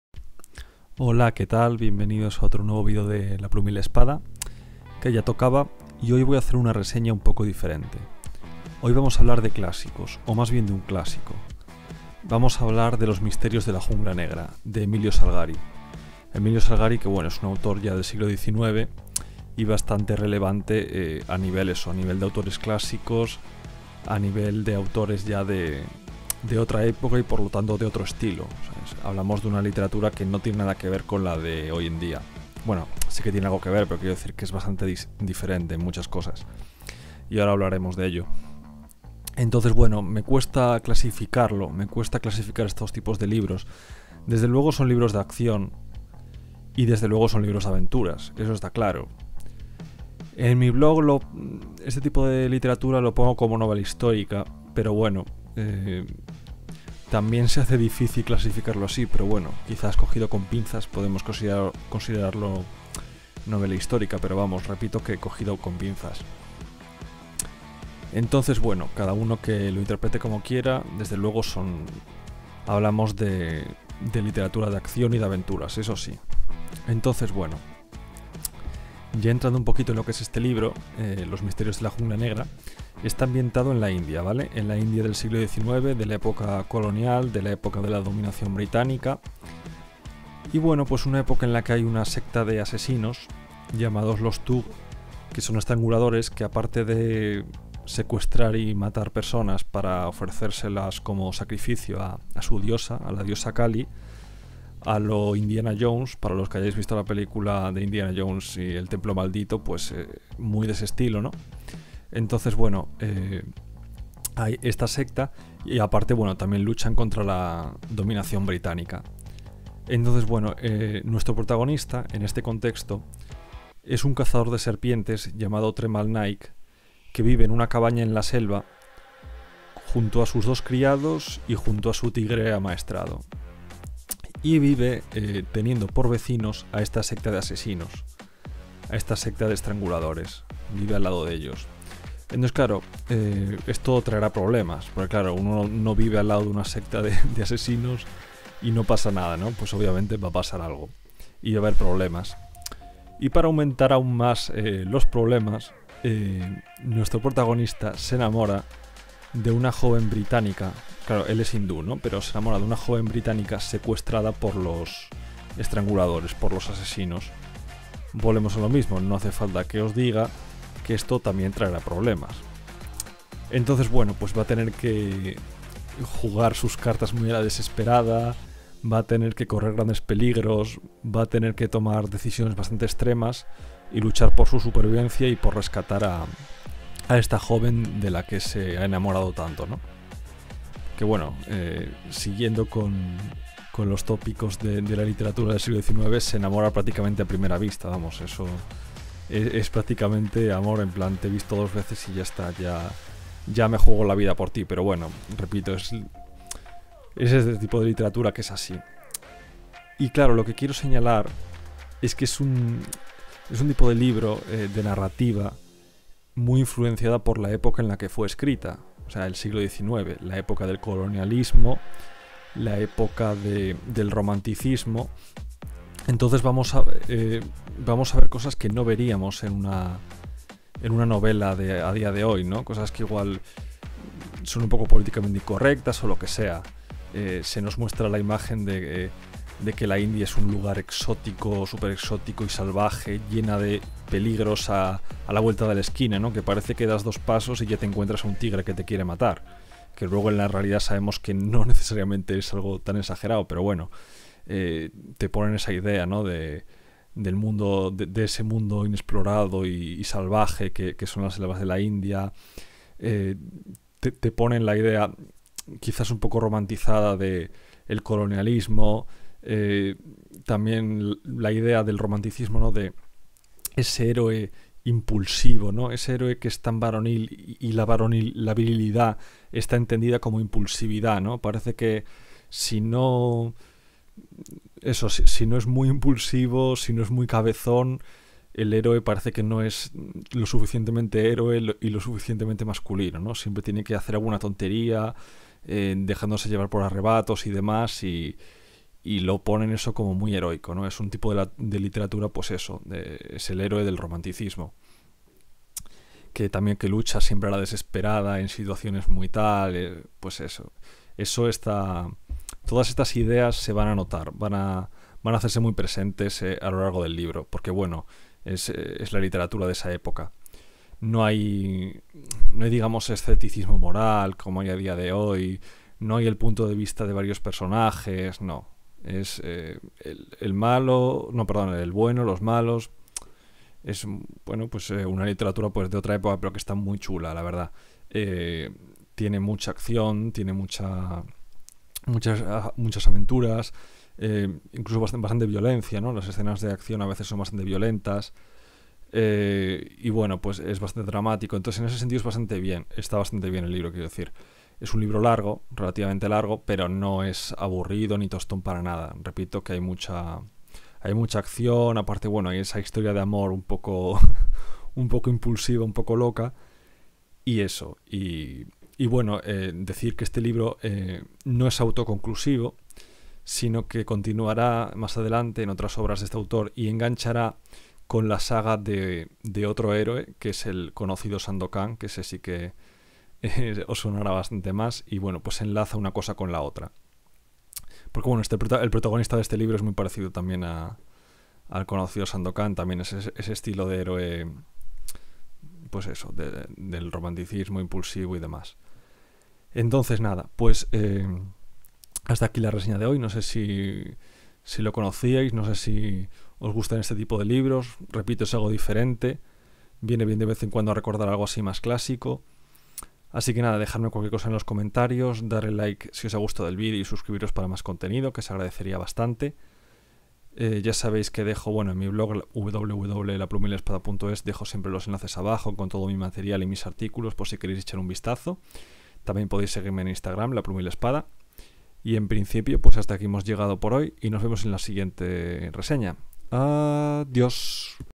🐅🐅Los MISTERIOS De La JUNGLA Negra por Emilio Salgari (reseña)
Hoy vamos con un poco de clásicos para cambiar.

Instagram: La Pluma y la Espada